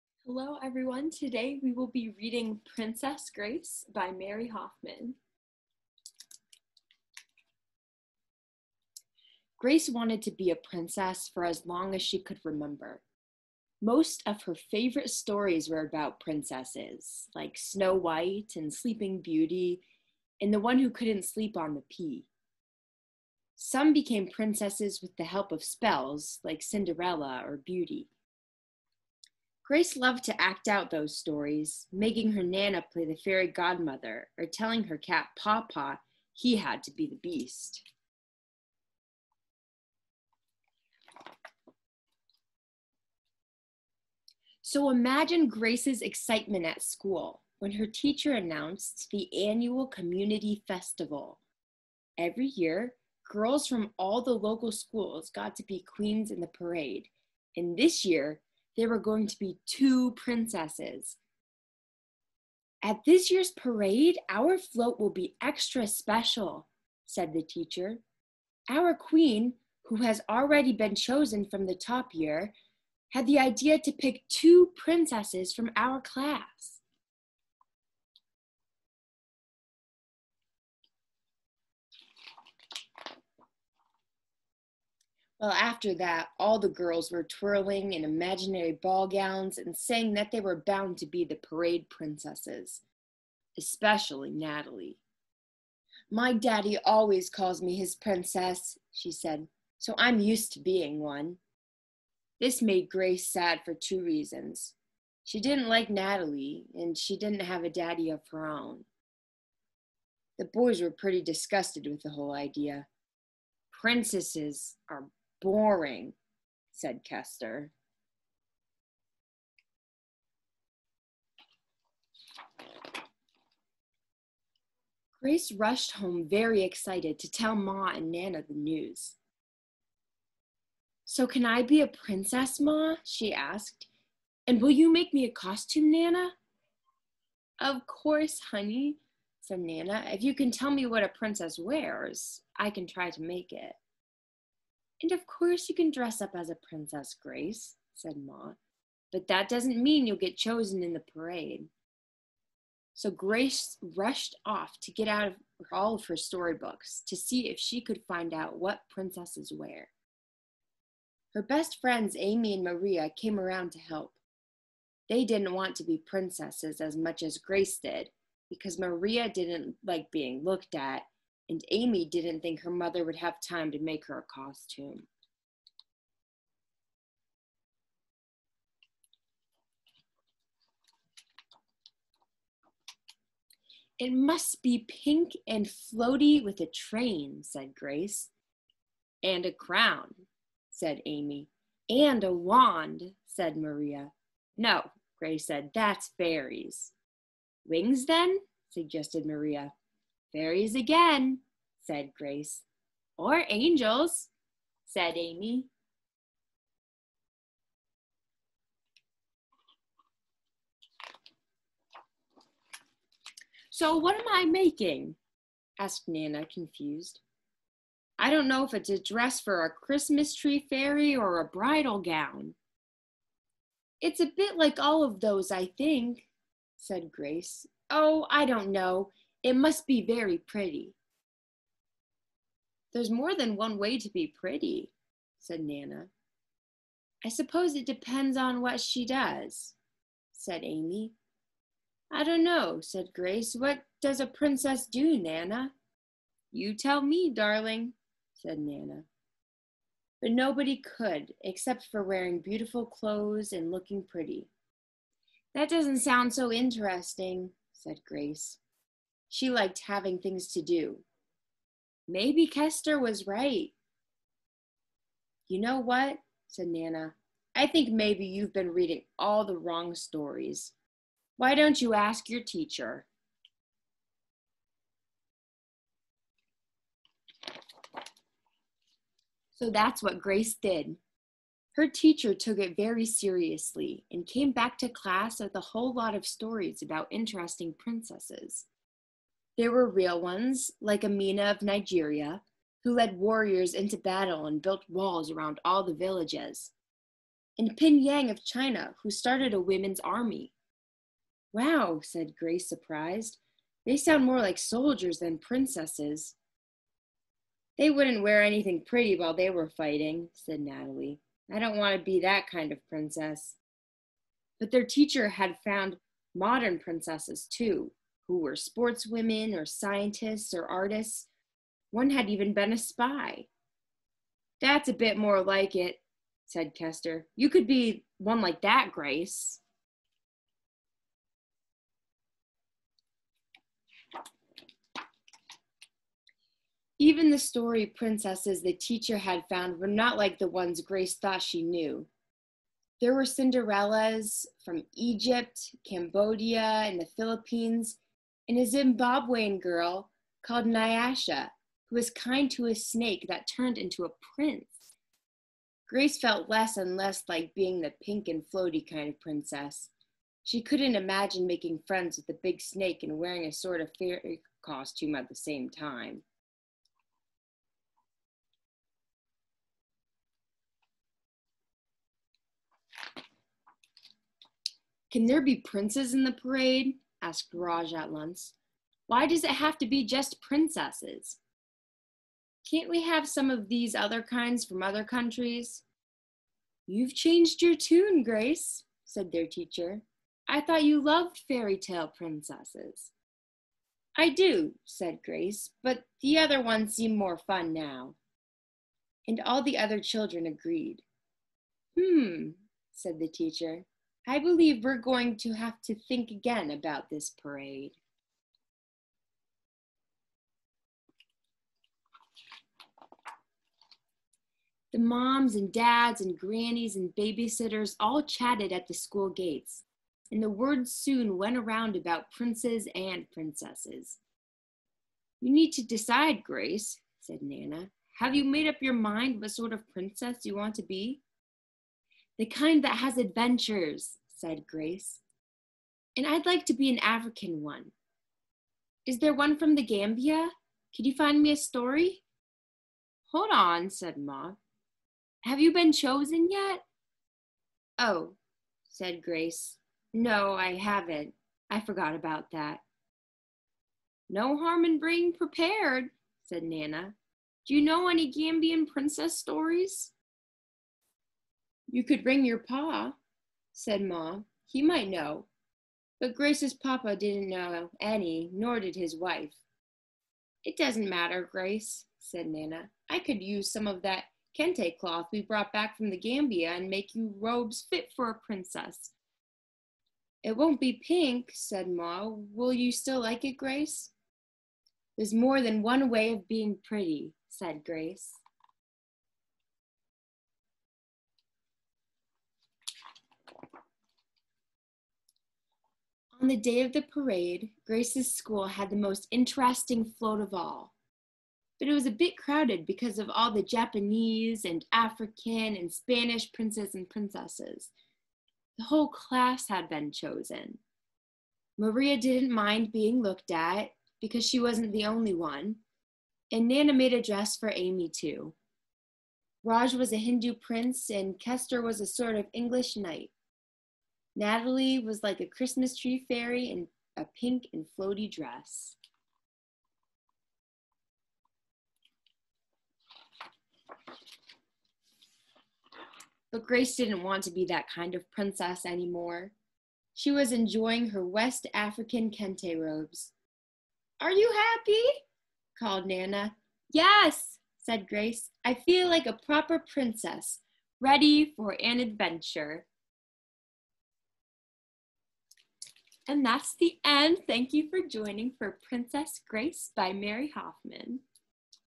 "Princess Grace" Reading
I hope you enjoy this reading of "Princess Grace" by Mary Hoffman.  Check out the book from your local library to follow along, or just watch the video!

All credit to the author, illustrators, publishing company, and all other contributors!